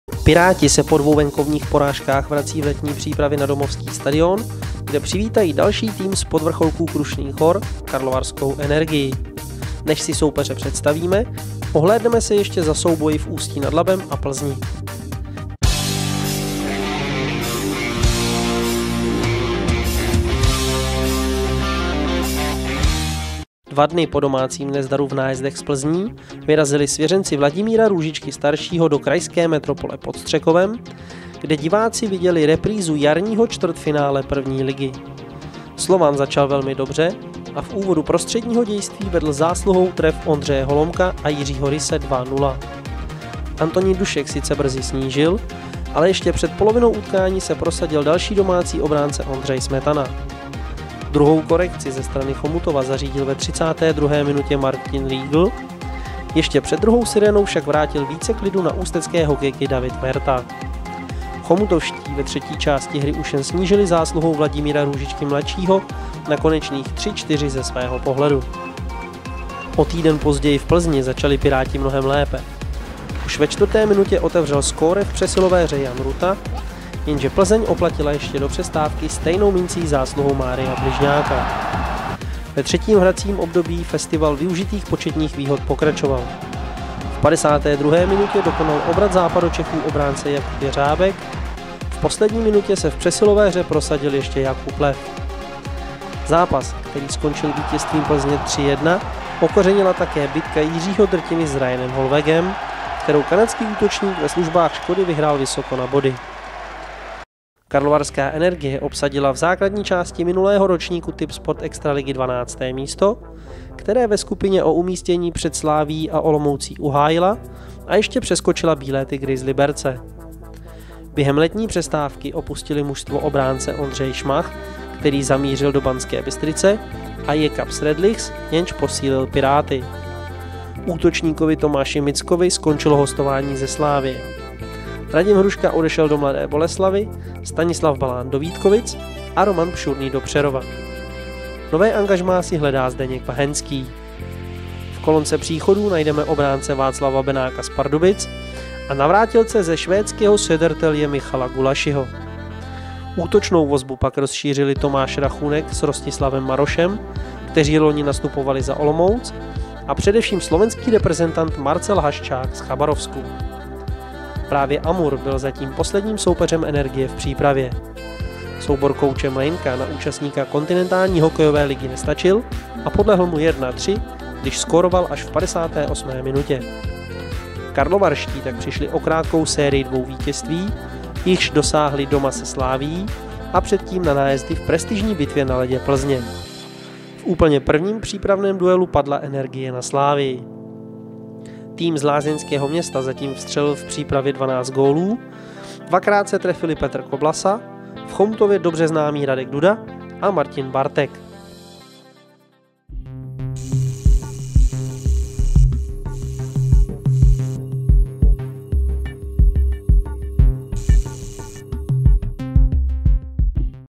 Preview - HC Energie Karlovy Vary
Před Piráty stojí jasný úkol, zkorigovat nepříznivou bilanci jedné výhry s ruským Omskem a tří proher s domácími soupeři. K pátému duelu nastoupí v chomutovské SD ARÉNĚ mistr Tipsport extraligy z roku 2009 HC Energie Karlovy Vary. Výsledky Energie jsou v přípravě jako na houpačce. Jak se jí bude dařit na severu Čech? Přijďte se podívat v úterý 18. srpna od 17:30.